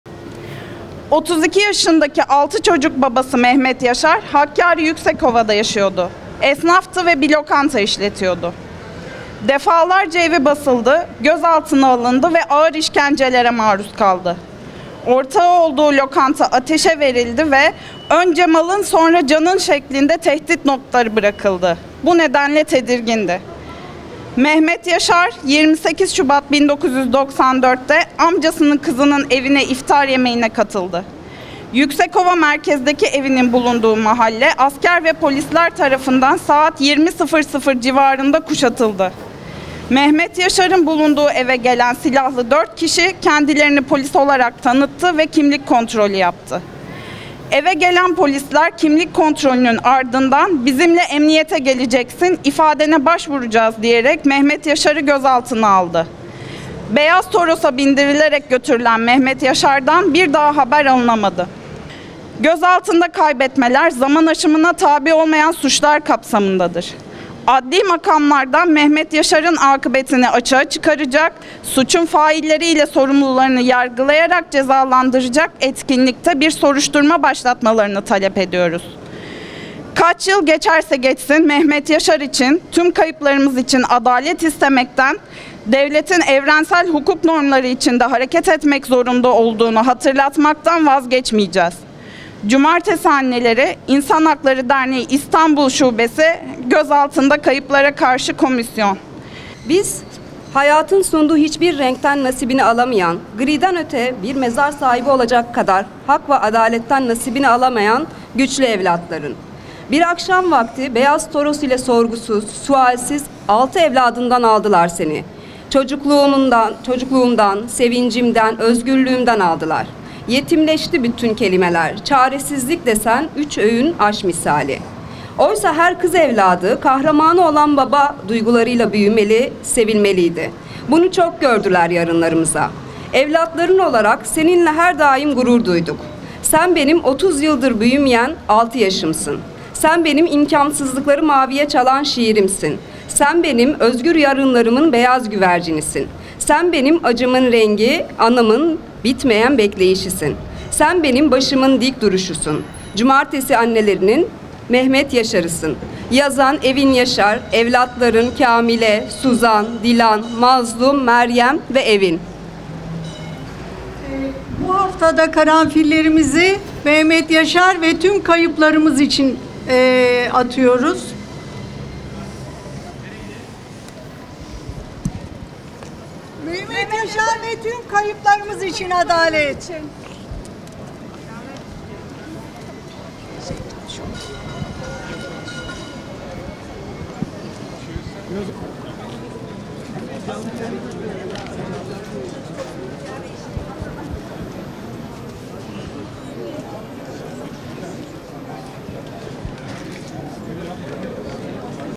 Cumartesi Anneleri, Mehmet Yaşar için adalet istedi
Cumartesi Anneleri, 30 yıl önce beyaz renkli Toros marka araca bindirilip gözaltında kaybedilen ve kendisinden bir daha haber alınamayan Mehmet Yaşar için adalet istedi.